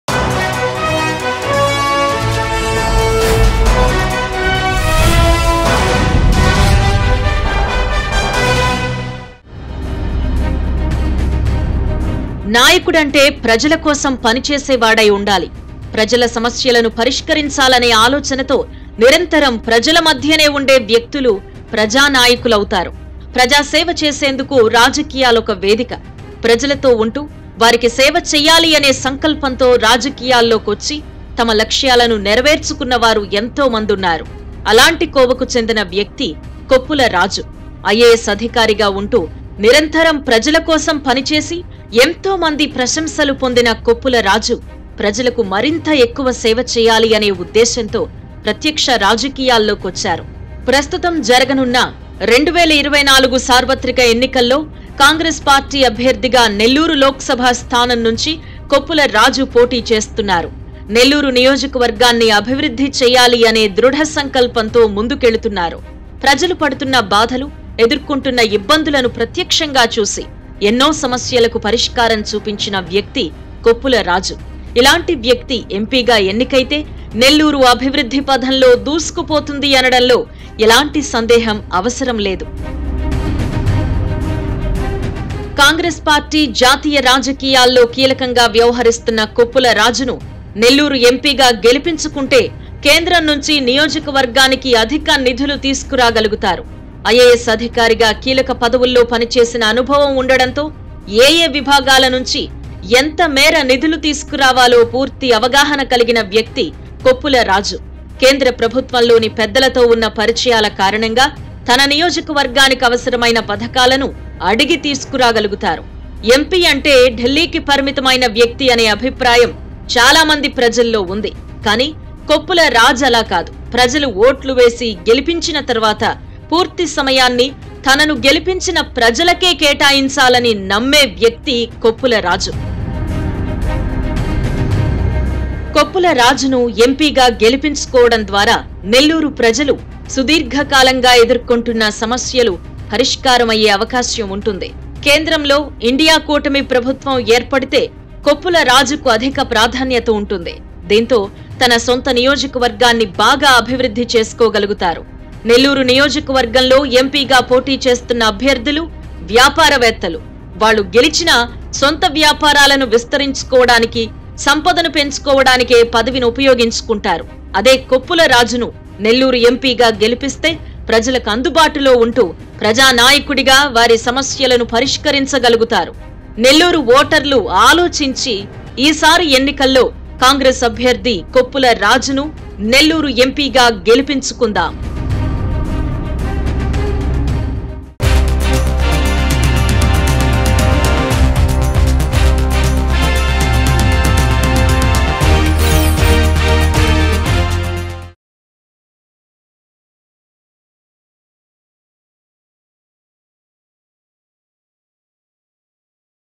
Special Story: Koppula Raju Political Report In Nellore | Congress MP Candidate | Mango News
Special Story: Koppula Raju Political Report In Nellore. Andhra Pradesh Elections To Be Held On May 13th, 2024.